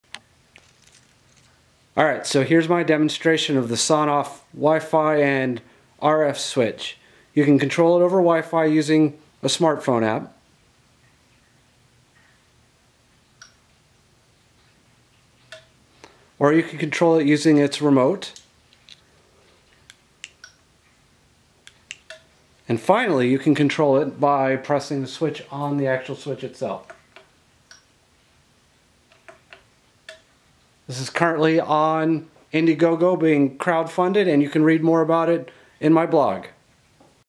Sonoff Demonstration
This is a demonstration of the Sonoff WiFi/RF electrical switch from ITEAD Studios.